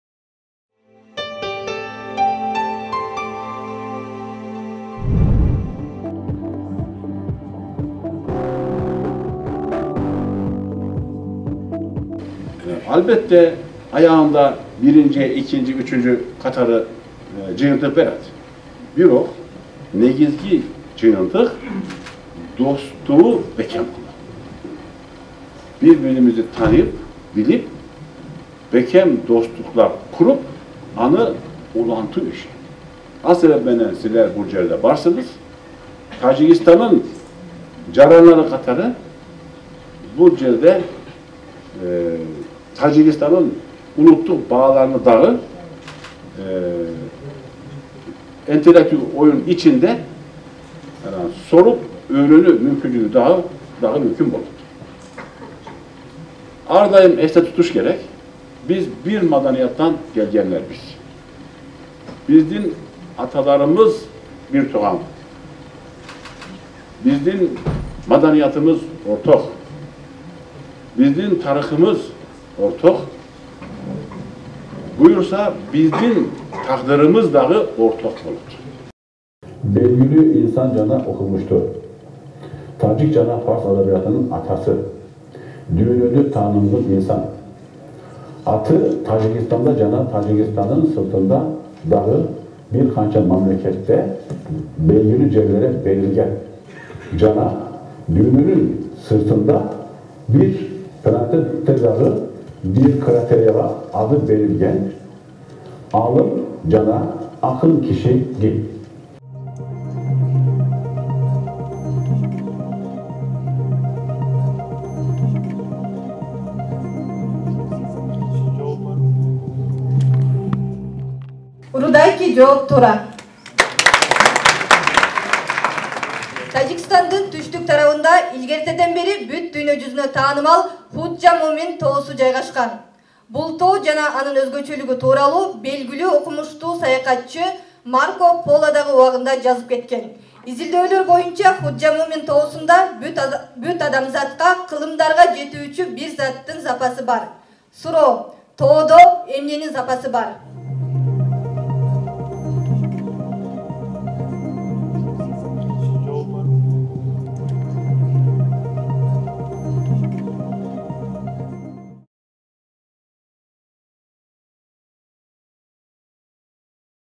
УЛУТТУК МУРАСТАР
КТМУда "ӨЛКӨЛӨРДҮН УЛУТТУК МУРАСТАРЫ" АТТУУ ИНТЕЛЛЕКТУАЛДЫК ОЮНДАР СЕРИЯСЫ УЮШТУРУЛУУДА